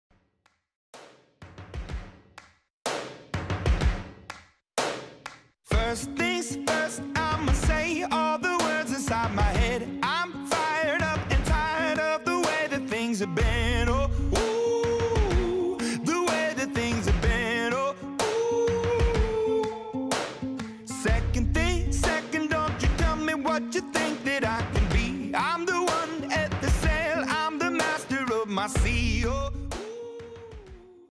🎸Kinetic Typography Sami Beltran Cueva Efectos Especiales Eliana Barrios🥁
Trabajo de Kinetic Typography realizado por nuestra estudiante Sami Beltrán en la clase de “Efectos Especiales”, con la profe Eliana Barrios . @eliana_barrios48💻👌🏼

¿Qué esperas para ser parte del Mundo Multimedia? 💡